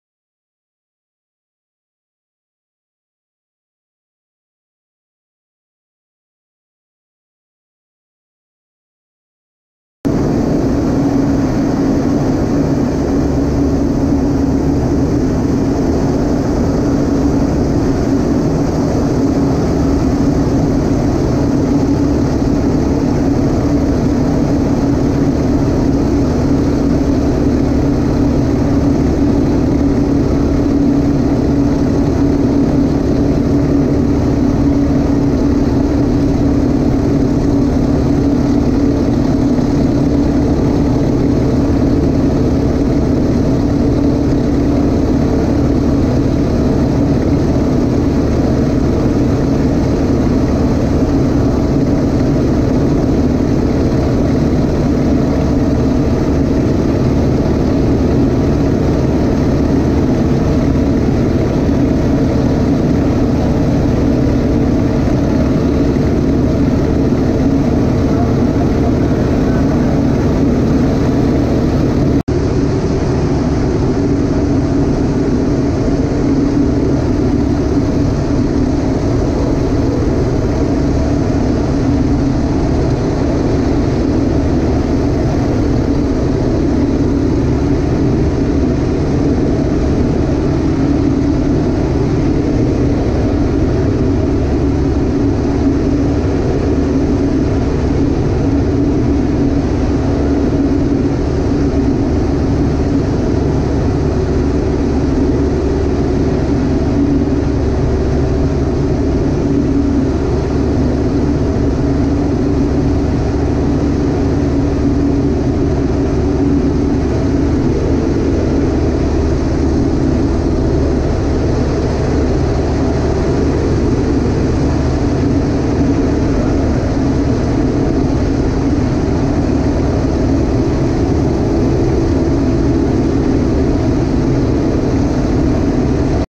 PZL M18B elafonisos fire 2019. Turning difficulties in firefighting mission due to strong wind.
Τhe video shows how the PZL airplane fly with water load 1800 kilos, on strong wind up to 40 Knots on a fire fighting mission. There are several turning difficulties  and turbulence in this firefighting mission.
For your info, mission accomplished and  water drop made to the ground, no matter the turbulence.
---video reuploaded after first upload---